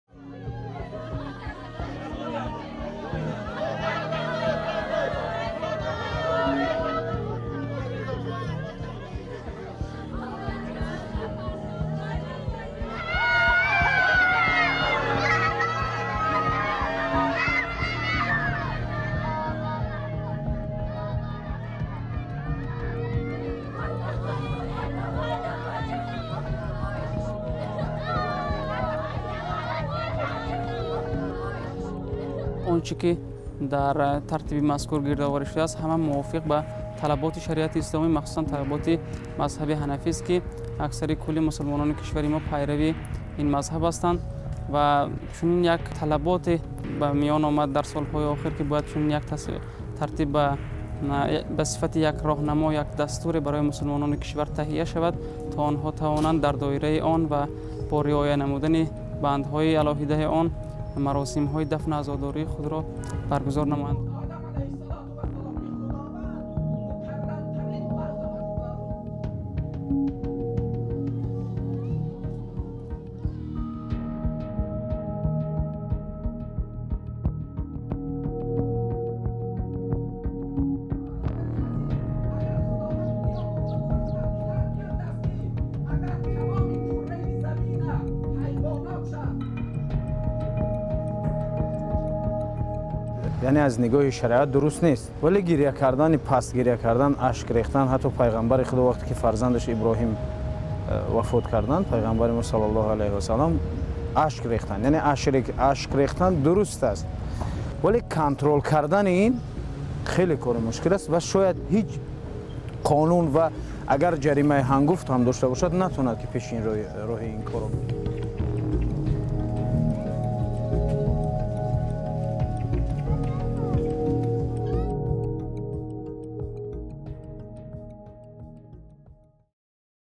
Tajikistan Clamps Down On Loud Crying At Funerals
Tajikistan has issued regulations on how mourners can behave at funerals, forbidding wearing black, pulling one's hair, or wailing too loudly. The measures are part of wide-ranging restrictions on cultural life, intended, officials say, to promote Tajik traditions and rein in religious extremism.